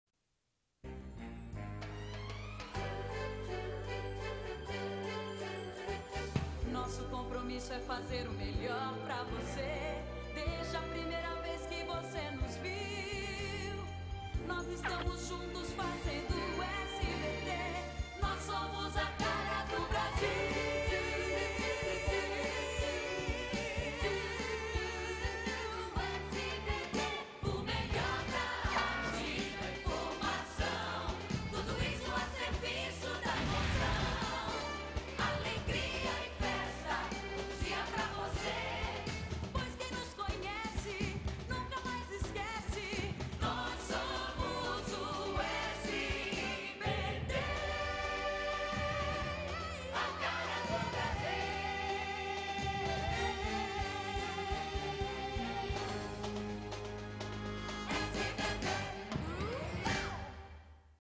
Vinheta A Cara do Brasil | SBT | 1998
Vinheta do SBT em 1998, no aniversário de 17 anos o canal voltou as origens criando atrações mais populares com "A Cara do Brasil". Também destacava imagens do CDT da Anhanguera inaugurado dois anos antes.